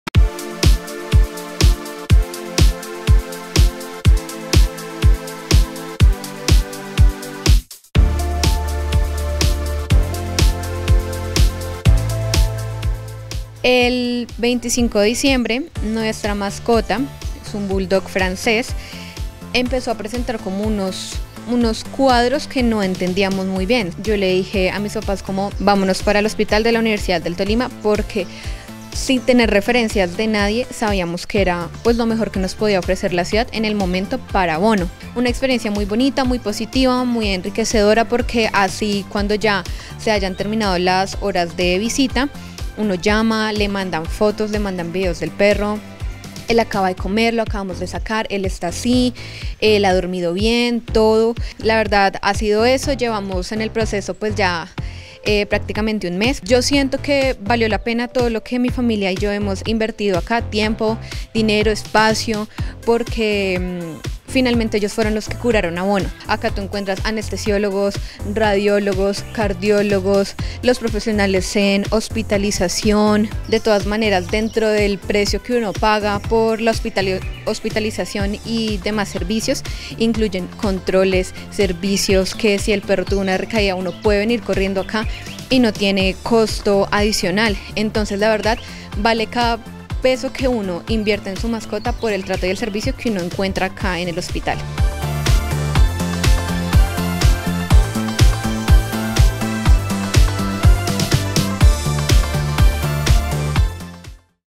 HVUT | Conoce la experiencia de Bono en nuestro hospital
Natalia Rojas nos cuenta su experiencia frente a la atención que le prestaron a su mascota Bono en el Hospital Veterinario de la Universidad del Tolima.